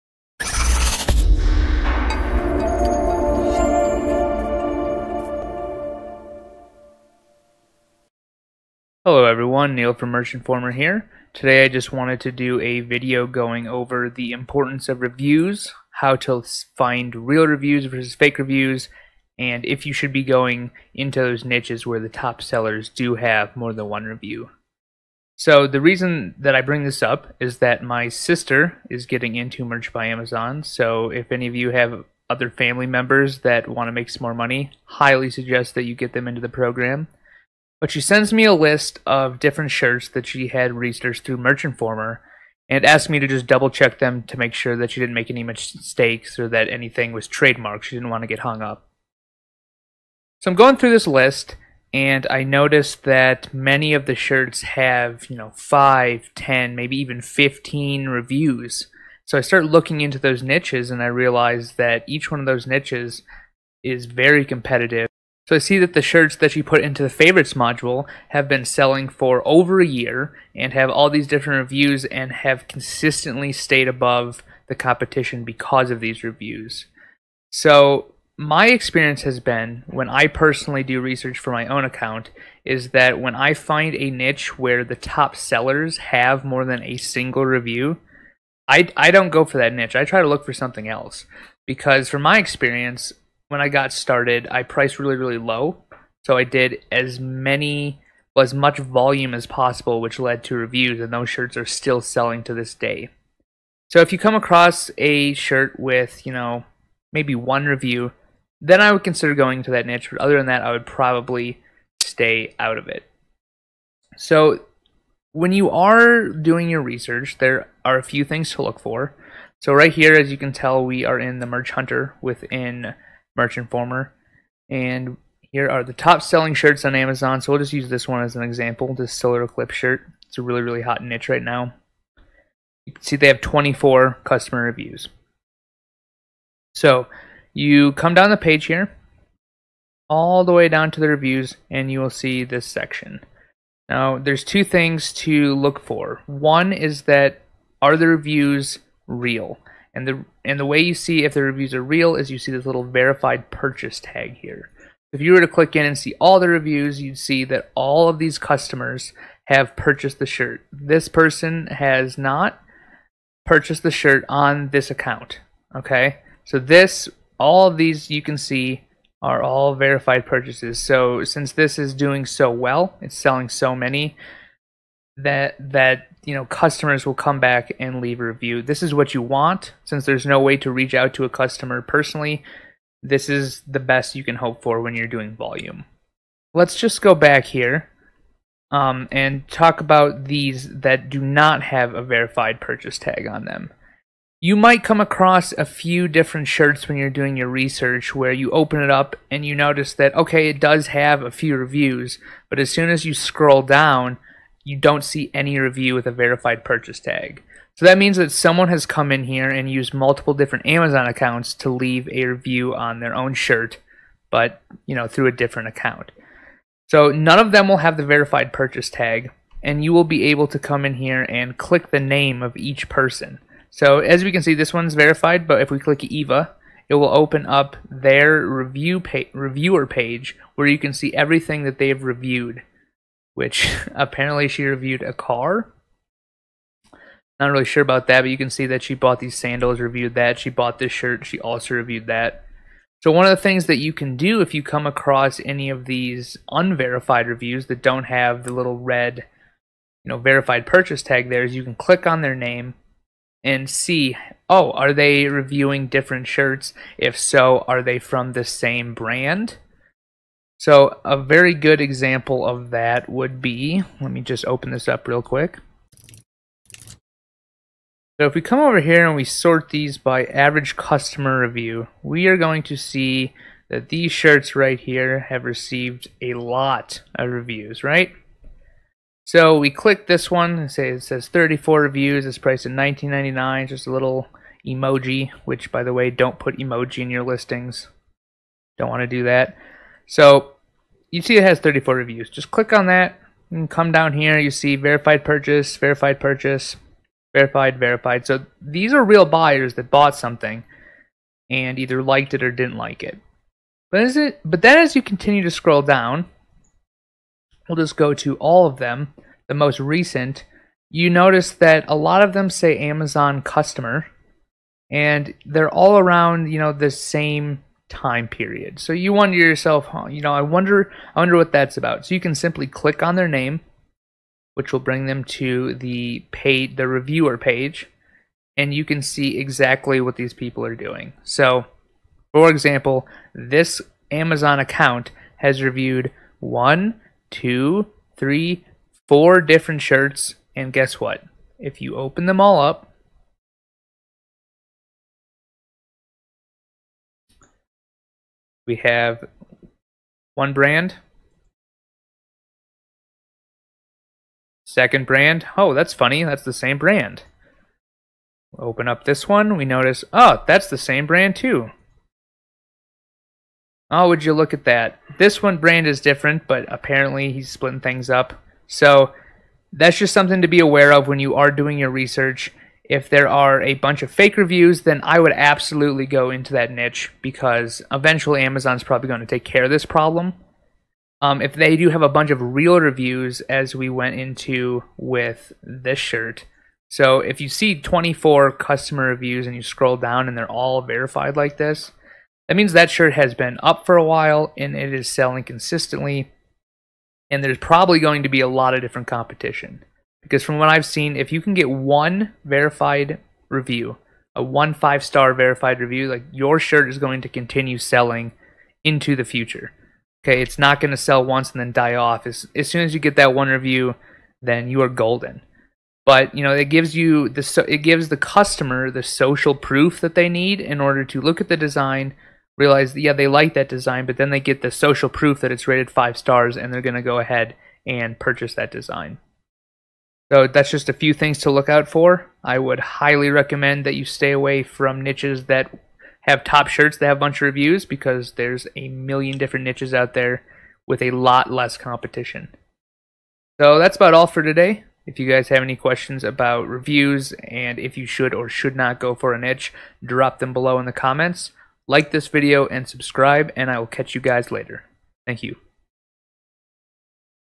Merch by Amazon Shirt Reviews. Do they matter and what to look for
When doing research for Merch by Amazon niches, a lot of people simply discount how important reviews really are. A single review can change the entire directory of a shirt!

Learn if you should go into these Amazon merch niches, what to look for when it comes for real reviews, and how to spot those shady people leaving fake reviews.

All is revealed in this video. Any questions, drop them below!